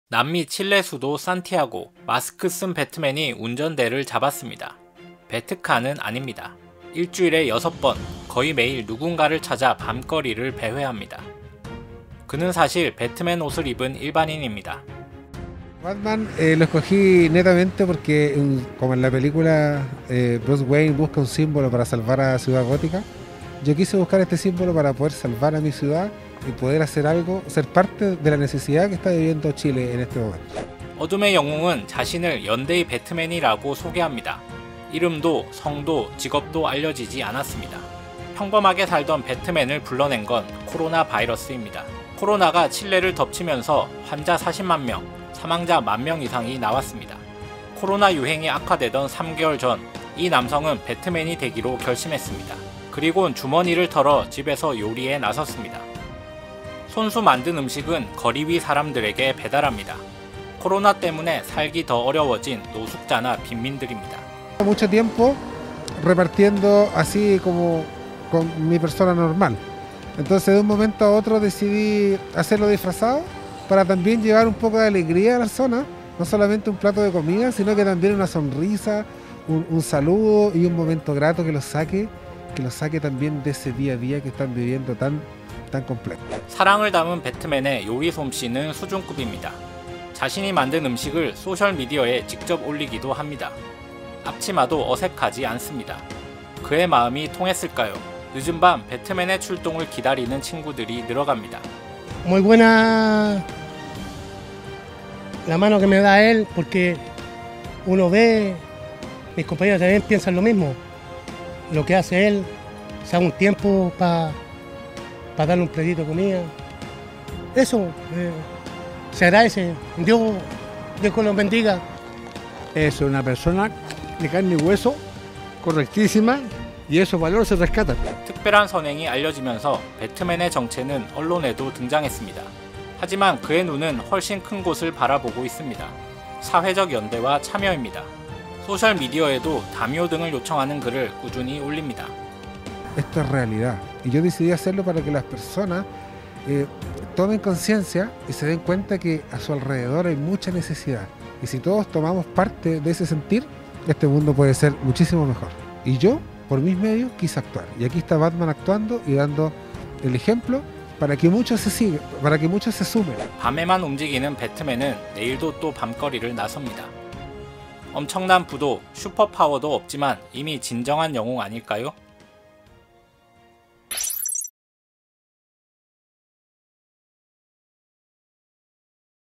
"코로나가 날 움직이게 했다" 밤거리 돌며 거리 위 이웃 지키는 배트맨의 정체
'어두운 밤에 움직이는 고독한 영웅'.

만화와 영화로 잘 알려진 배트맨의 이미지다. 배트맨이 최근 고담시가 아닌 남미 칠레의 수도 산티아고에 나타났다. 영어가 아닌 스페인어를 쓴다는 점 외에 외형상 크게 다른 건 없다. 그는 스스로를 '연대의 배트맨'(Batman solidario)이라고 부른다.

CNN 등에 따르면 이 배트맨은 신종 코로나바이러스 감염증(코로나19) 때문에 모습을 드러냈다. 앞치마와 마스크를 착용한다는 점 외엔 외형상 크게 다를 바 없는 '연대의 배트맨' 그는 왜 매일 밤거리를 헤맬까?